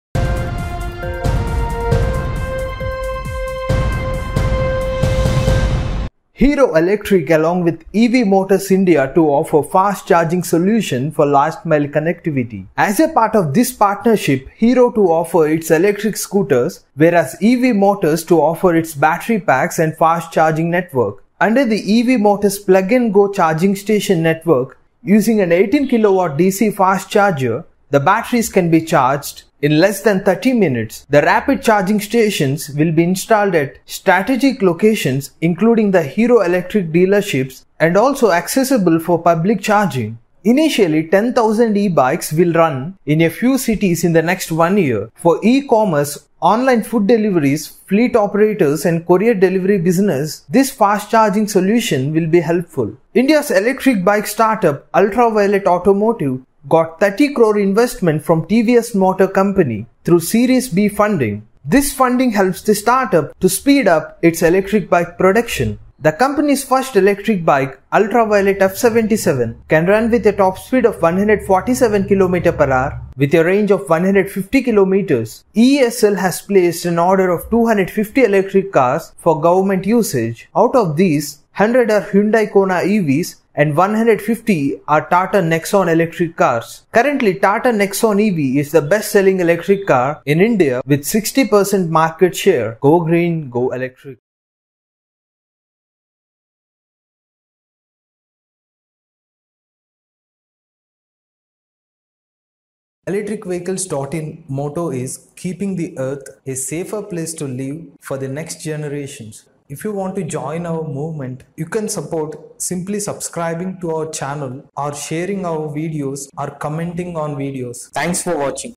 Hero Electric Scooters Charge in 30 Minutes - EV News 112
Electric Vehicles News Today
1.Charge in 30 Minutes Hero Electric Scooters with EV motors India
2. Ultraviolette Automotive has raised Rs 30 crore investment
3. Electric Cars for Govt Use EESL